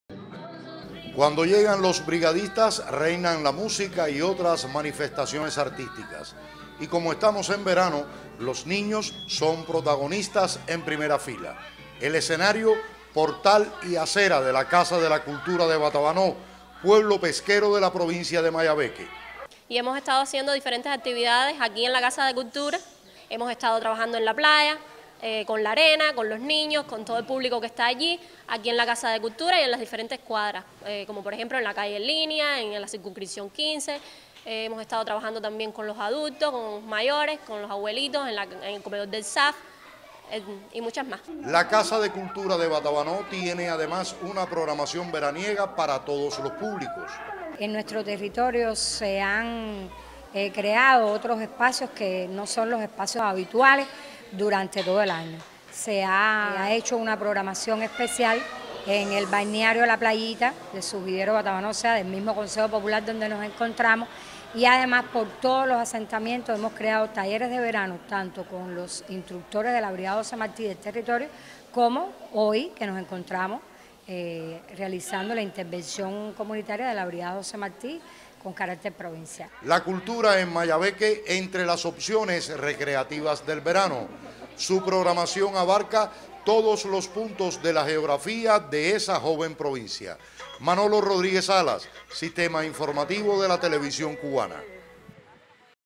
NTV CULTURA DE MAYABEQUE EN FUNCIÓN DEL VERANO
LA BRIGADA DE INSTRUCTORES DE ARTE JOSÉ MARTÍ, MOVIMIENTO CREADO POR FIDEL EL 20 DE OCTUBRE DEL AÑO 2004, MANTIENE SU ESENCIA FUNDACIONAL DE PROMOVER  EL TALENTO ARTÍSTICO EN TODAS LAS MANIFESTACIONES Y EDADES…POR ESTOS DÍAS, LA BRIGADA DE LA PROVINCIA DE MAYABEQUE  RINDE HOIMENAJE A SU FUNDADOR CON UN AMPLIO PROGRAMA DE ACTIVIDADES EN EL MUNICIPIO BATABANÓ, AL SUR DE ESA PROVINCIA…
GENERADORES.
1- SARAY MOREJÓN PÉREZ, INSTRUCTORA DE ARTE Y PRESIDENTA DE LA BRIGADA PROVINCIAL JOSÉ MARTÍ, MAYABEQUE.
2- MARÍA DEL CARMEN DÍAZ GARCÍA, DIRECTORA MUNICIPAL DE CULTURA DE BATABANÓ